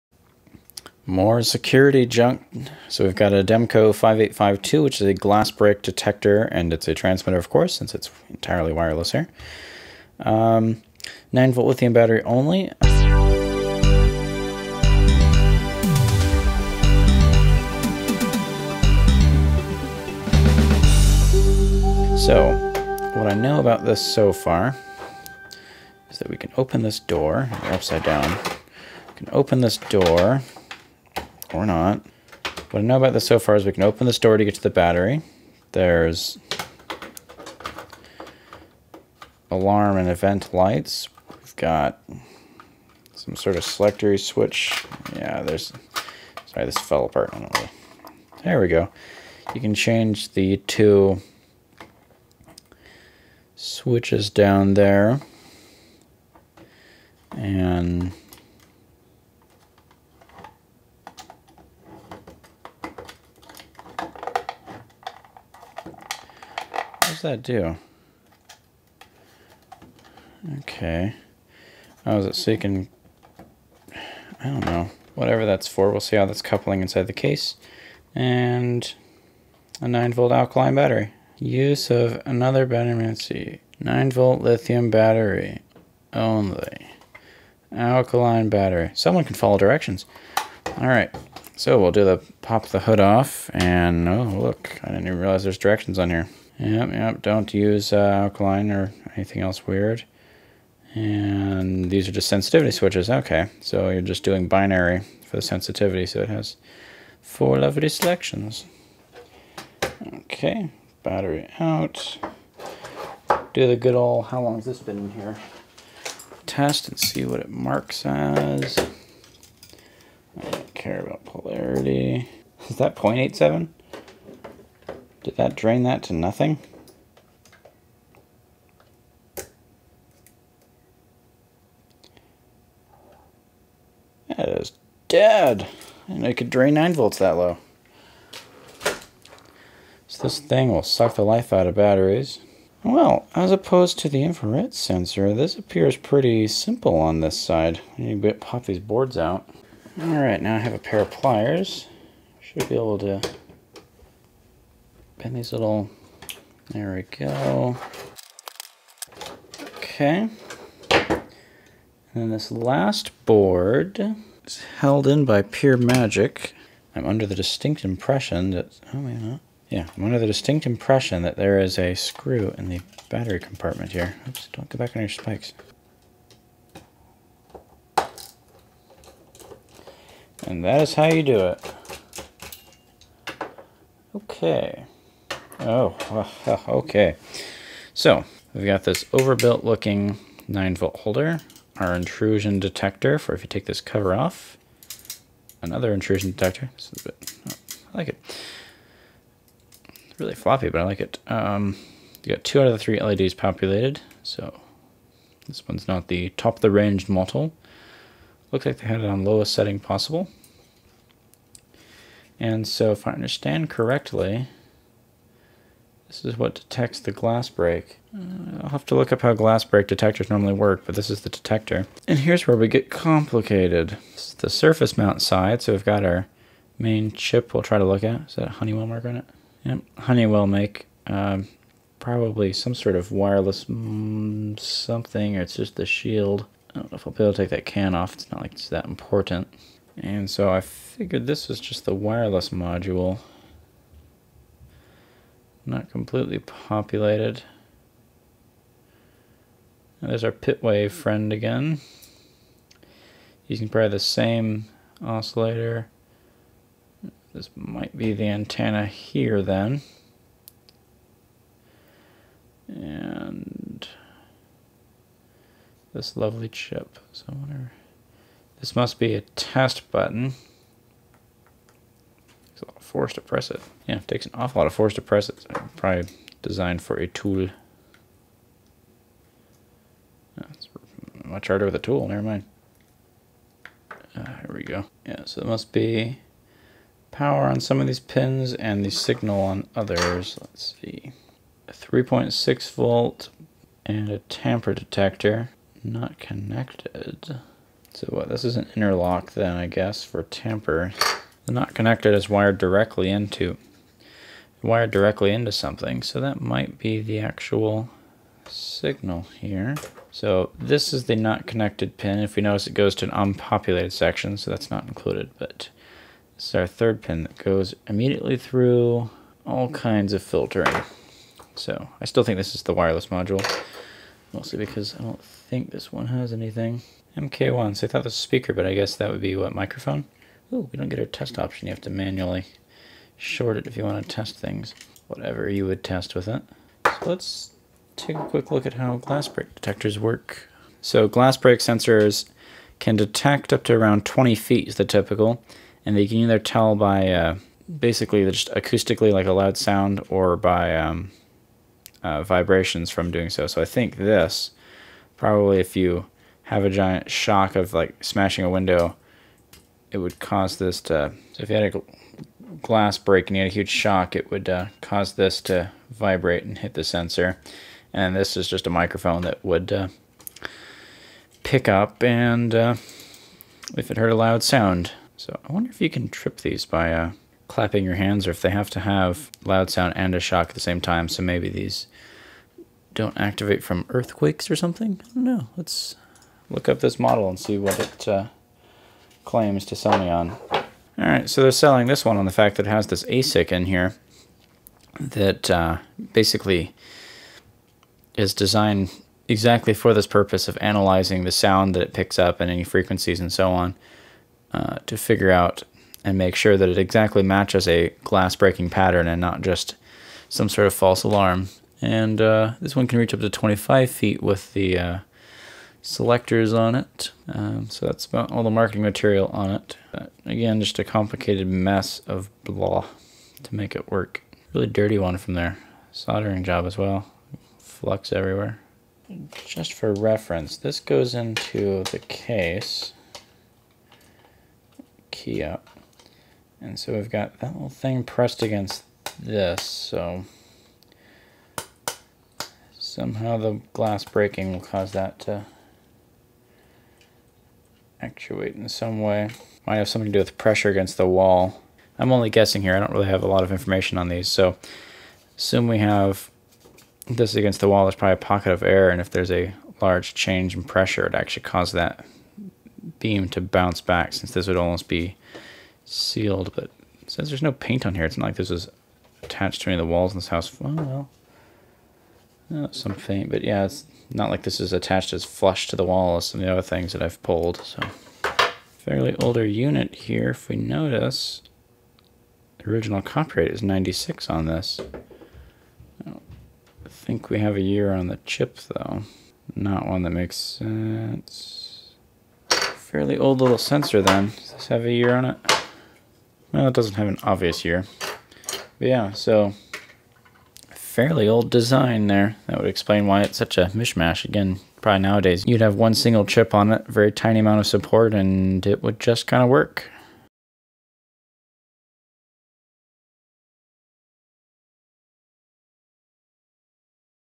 Glass Break Detector Teardown
Teardown of a fairly old Ademco 5852 Glass Break Detector and Transmitter. Designed so if someone shatters a window, it trips the alarm.